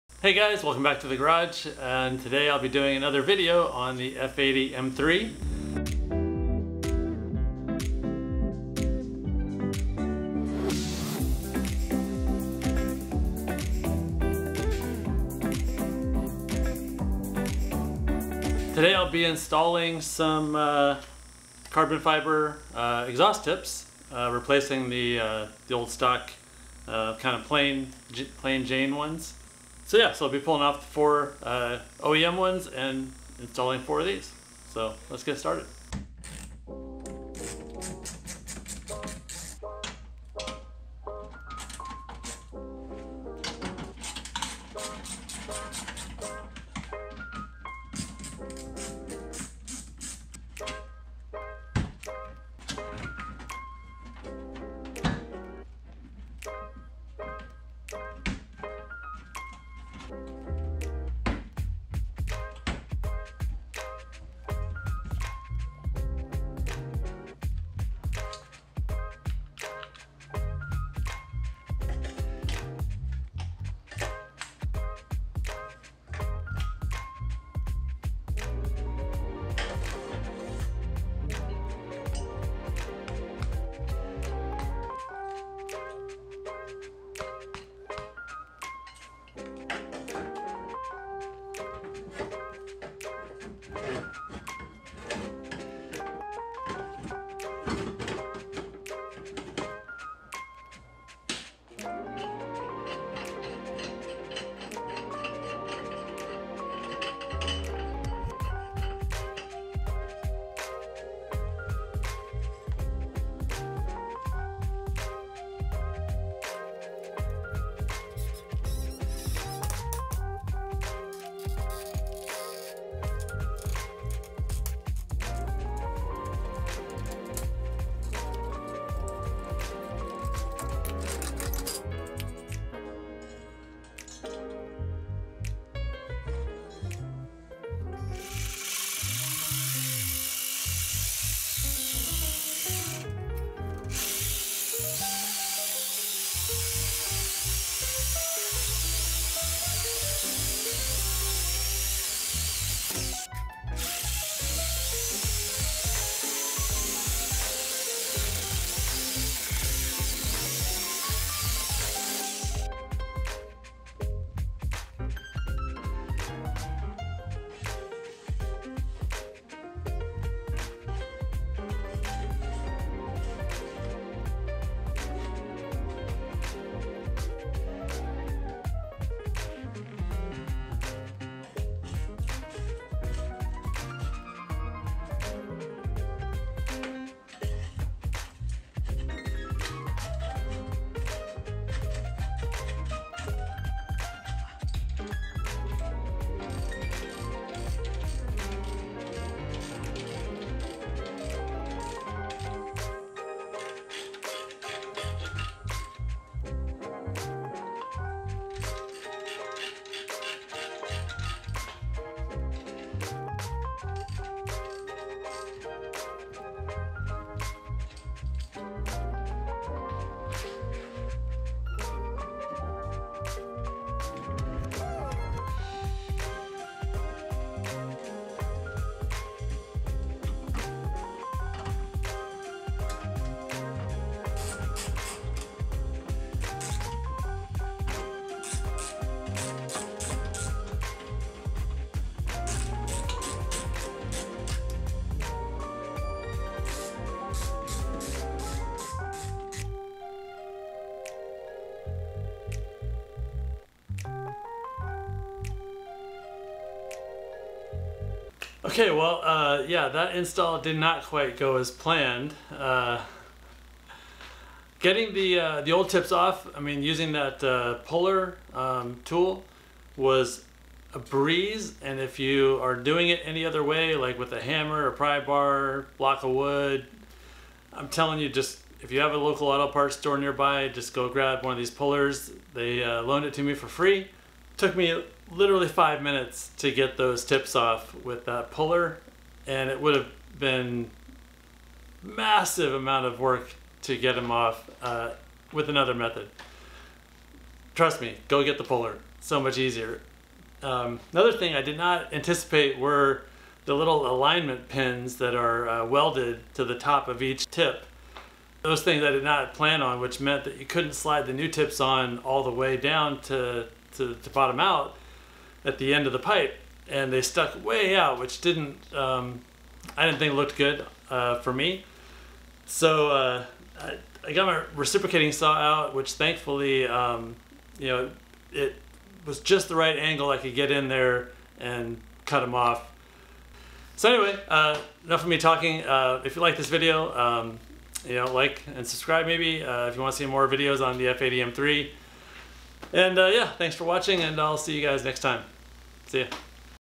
How to install M3 carbon exhaust tips
Quick video on how to install carbon exhaust tips on my F80 M3. Including a quick and easy way to get the old tips off without breaking your knuckles.